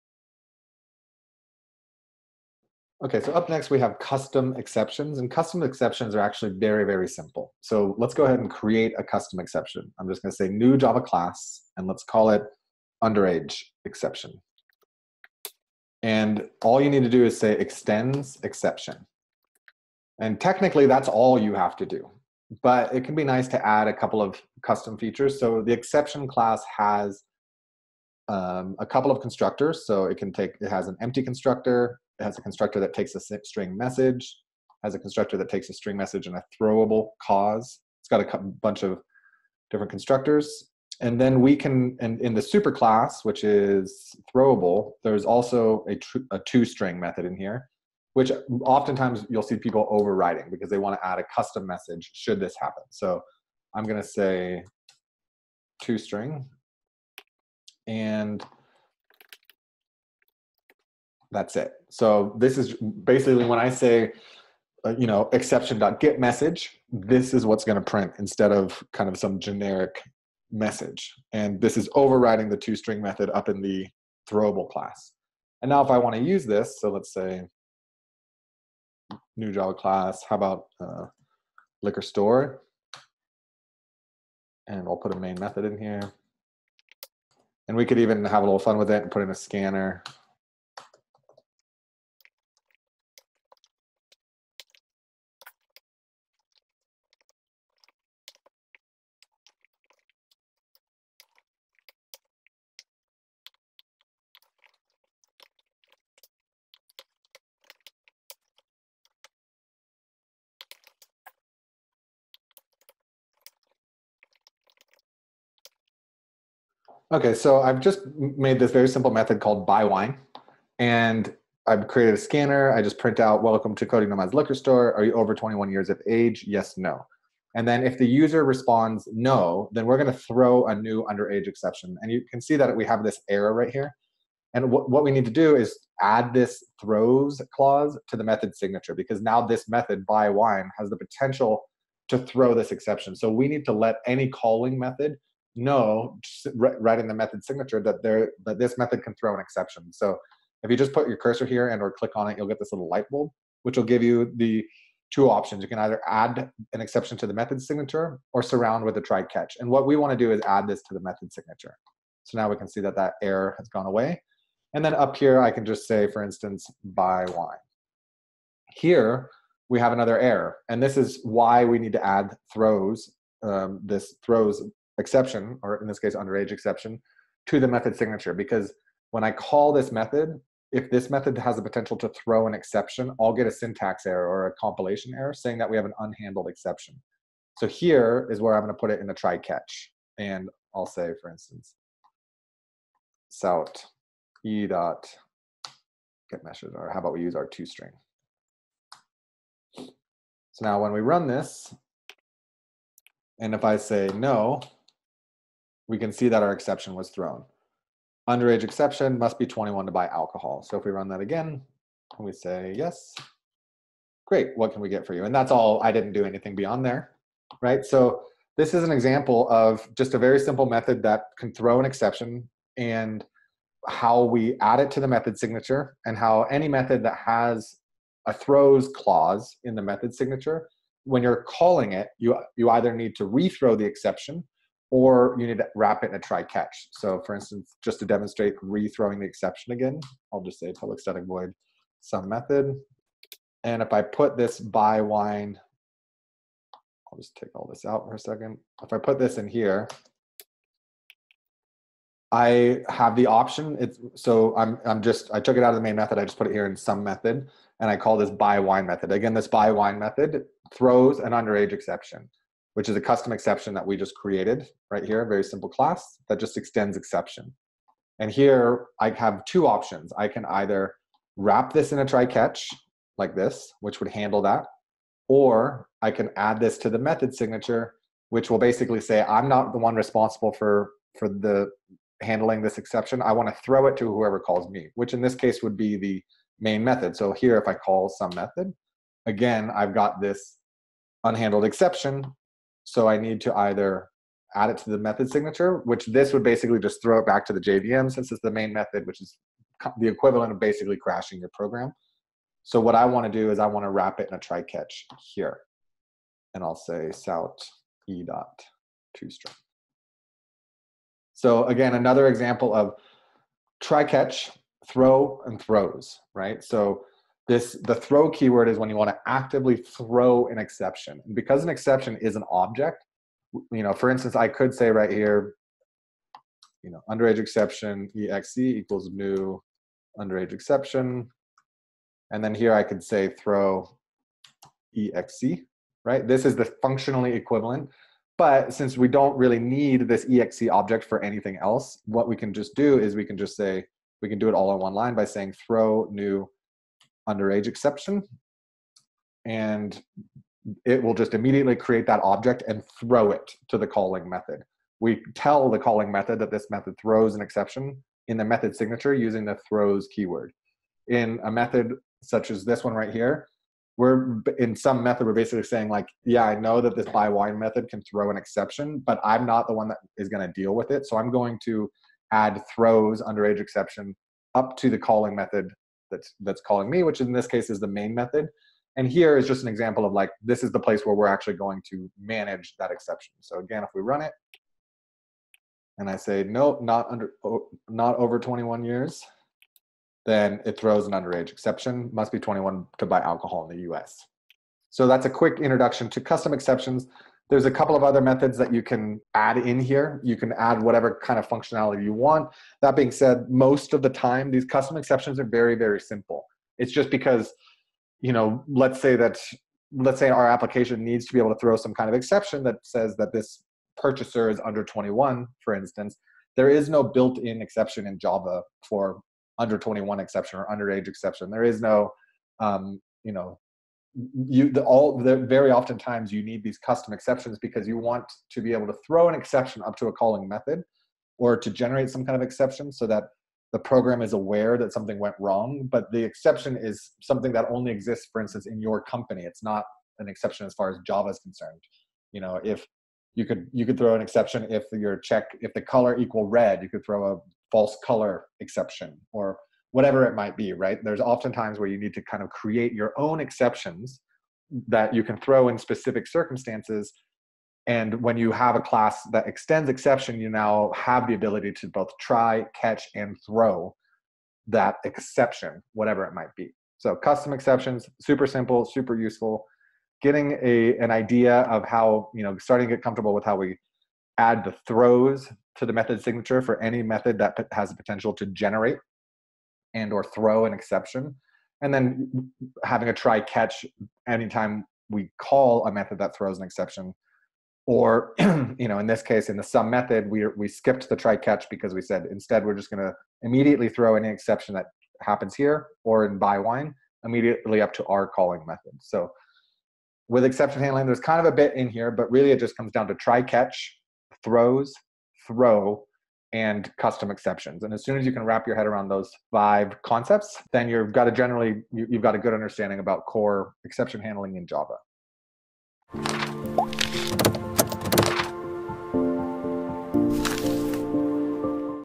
Java Exception Handling Part 4: Custom Exceptions
In this tutorial we show you custom exceptions, which are very simple.